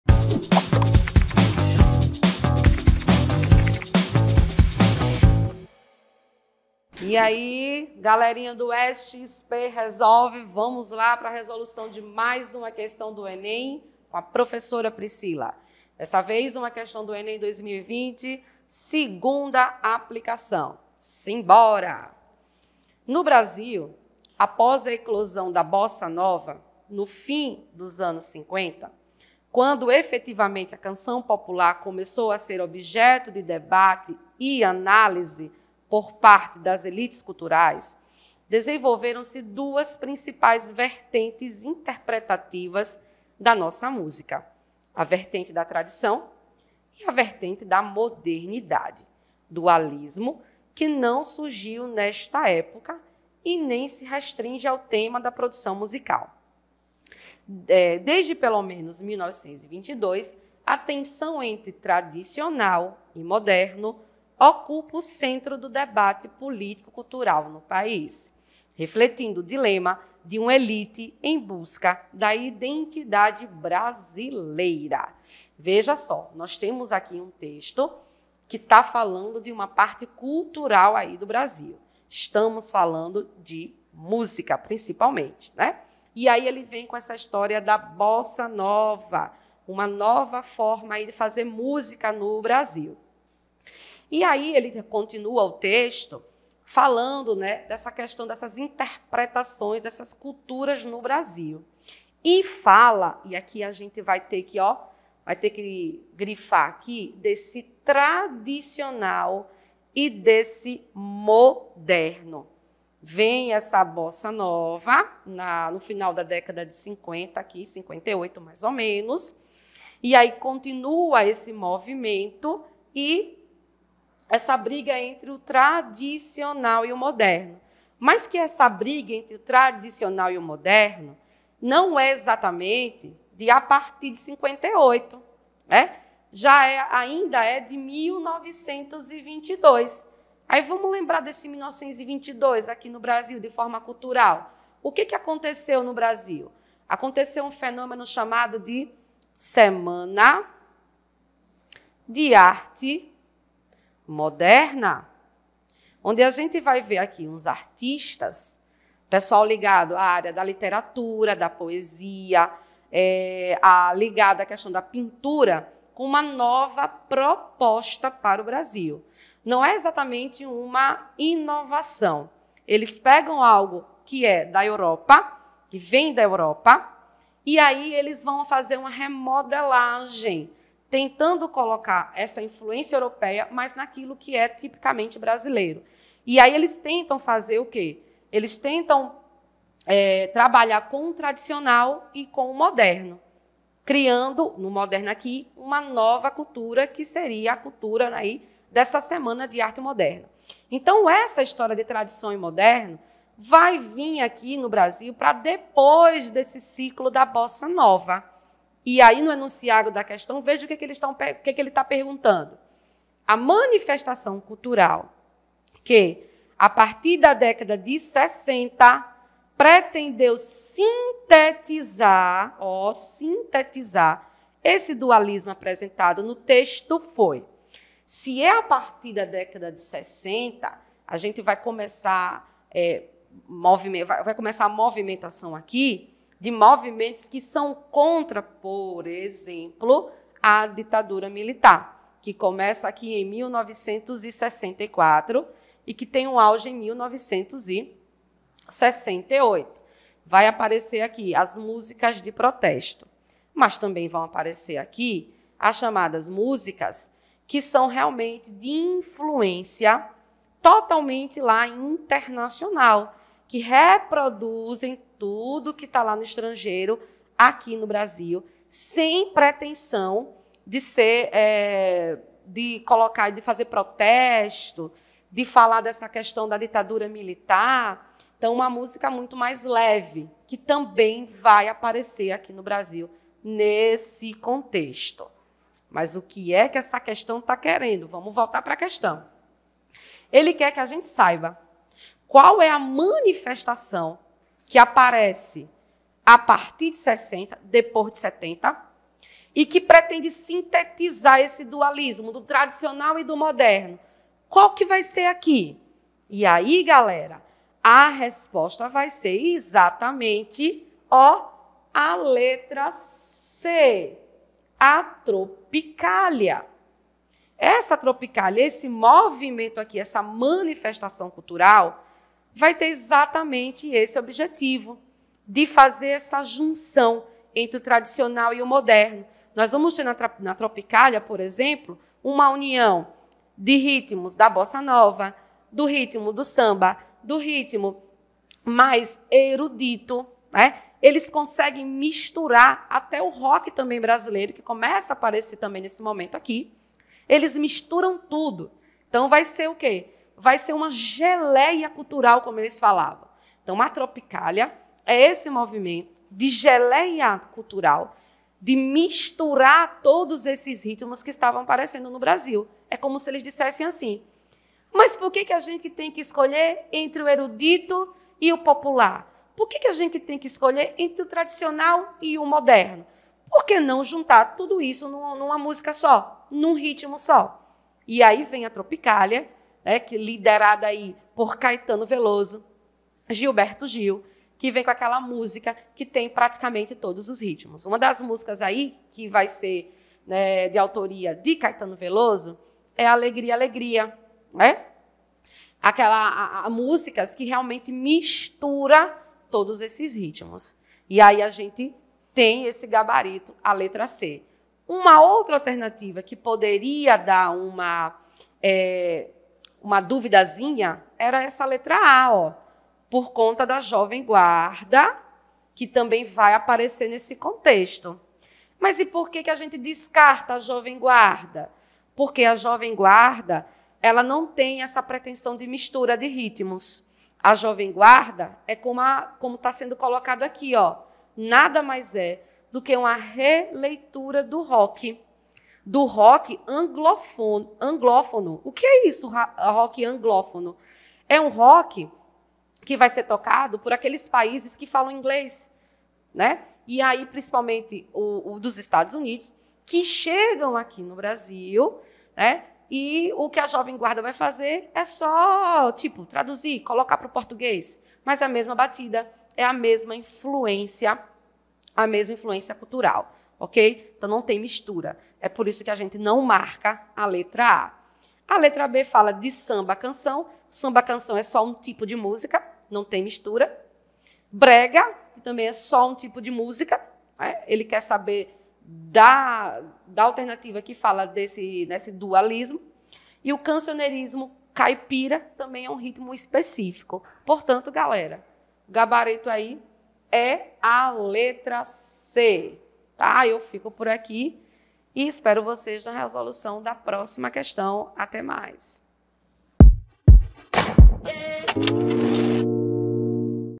NO BRASIL, APÓS A ECLOSÃO DA BOSSA NOVA, NO FIM DOS ANOS 1950 — QUANDO (...) | DITADURA MILITAR
Encontre a sua questão nos seguintes cadernos:

NO NOSSO MANUAL DE SOBREVIVÊNCIA:
ASSUNTO: 3 REPÚBLICA | 3.4 DITADURA MILITAR

(ENEM 2020 2º APLICAÇÃO) No Brasil, após a eclosão da Bossa
Nova, no fim dos anos 1950 — quando efetivamente a canção
popular começou a ser objeto de debate e análise por parte das
elites culturais — desenvolveram-se duas principais vertentes
vertente da modernidade, dualismo que não surgiu nesta época
e nem se restringe ao tema da produção musical. Desde pelo
menos 1922, a tensão entre “tradicional” e “moderno” ocupa o
centro do debate político-cultural no país, refletindo o dilema de
uma elite em busca da identidade brasileira.
ARAÚJO, P. C. Eu não sou cachorro, não. Rio de Janeiro: Record, 2013.

A manifestação cultural que, a partir da década de 1960,
pretendeu sintetizar o dualismo apresentado no texto foi:

A. Jovem Guarda, releitura do rock anglófono com letras em
português.
B. Samba-canção, combinação de ritmos africanos com tons de boleros.
C. Tropicália, junção da música pop internacional com ritmos nacionais.
D. Brega, amostra do dia a dia dos setores populares com temas
românticos.
E. Cancioneiro caipira, retrato do cotidiano do homem do campo
com melodias tristes.